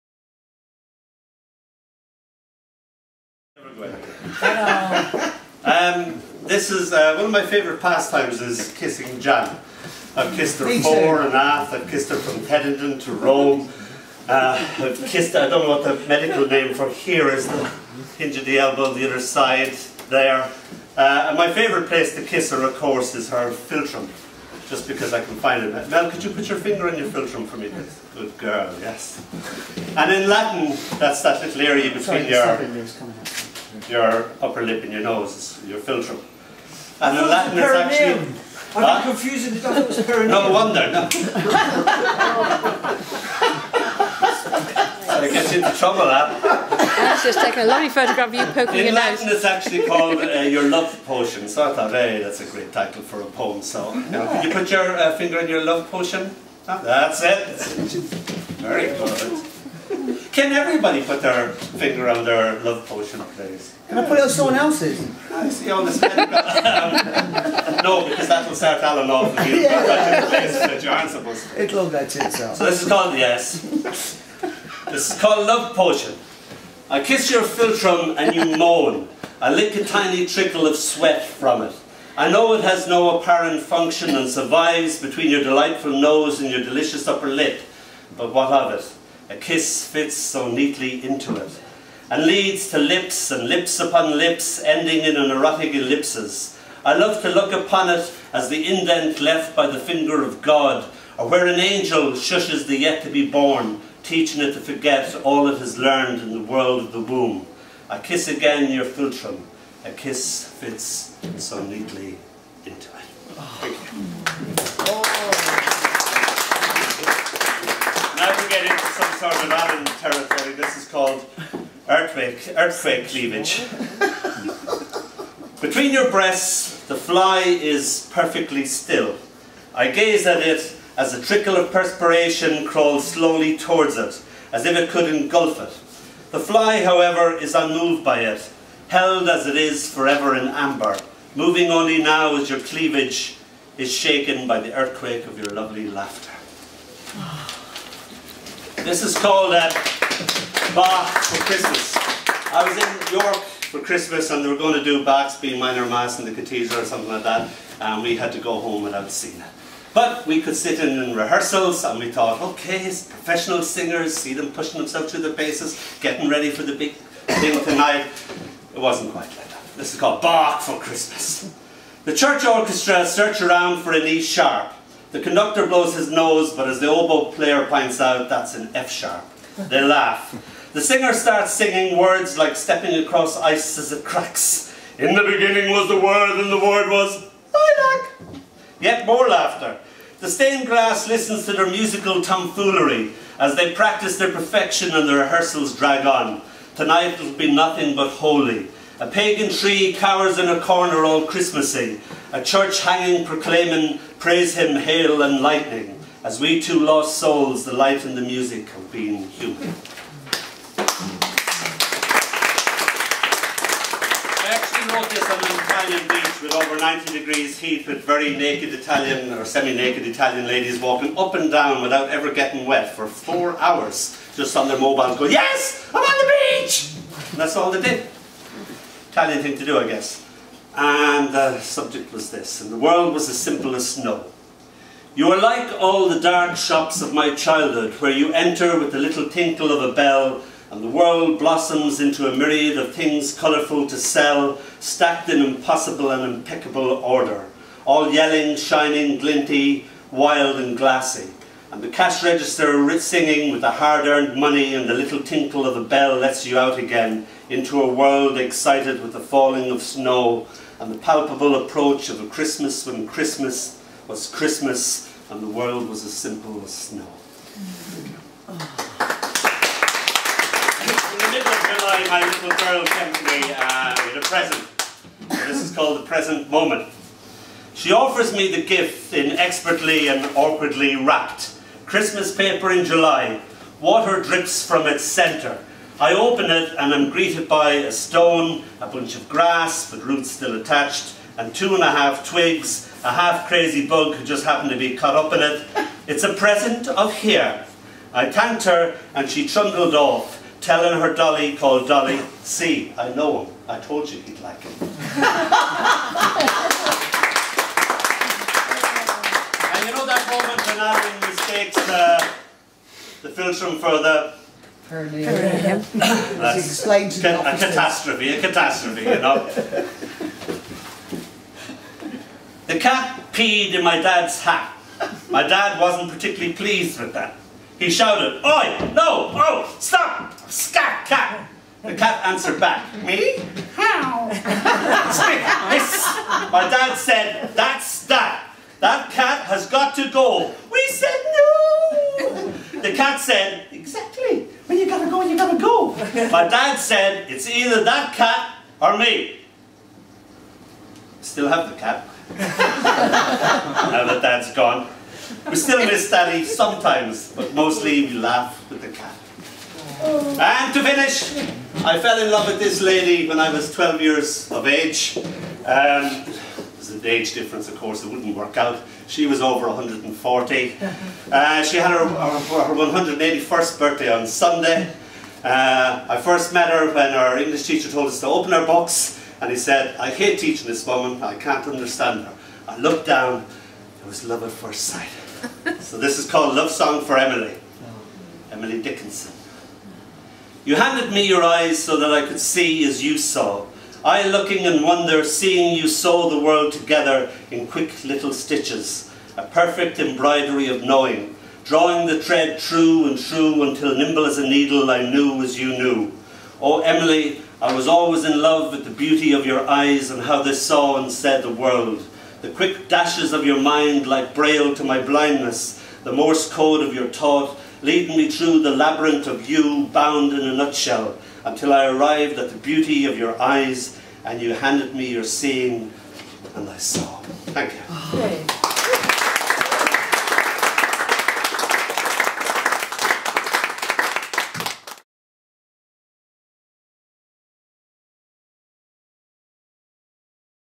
Kiss the Sky 14th Dec 2011
Dónall Dempsey reading at Kiss the Sky in Hampstead on 14th December 2011